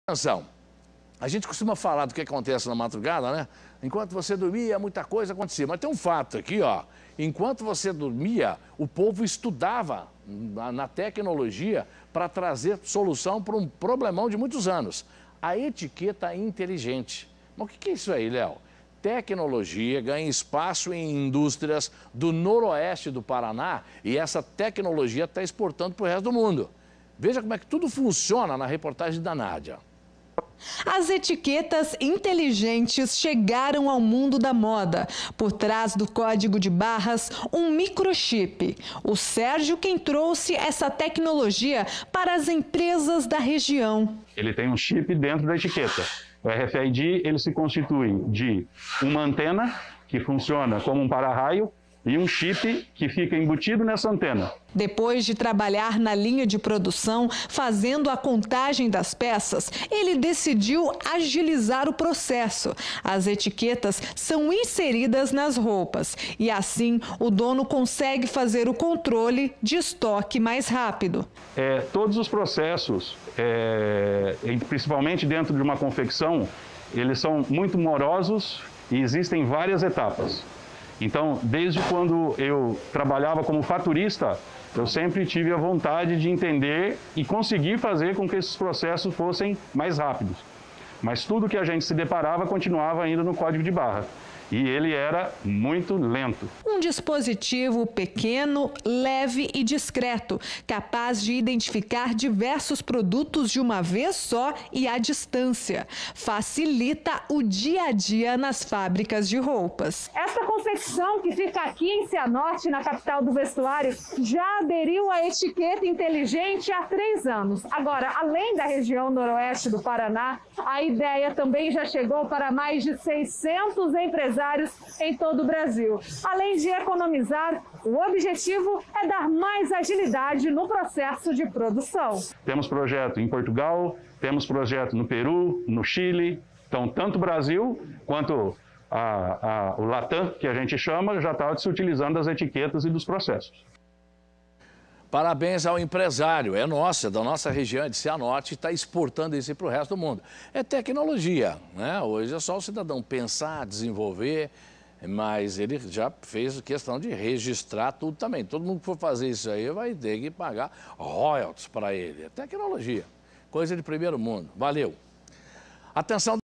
Nasce em Cianorte no PR uma tecnologia capaz de revolucionar a vida de fábricas e lojas.
O controle de estoque é feito em questão de segundo graças a descoberta inteligente.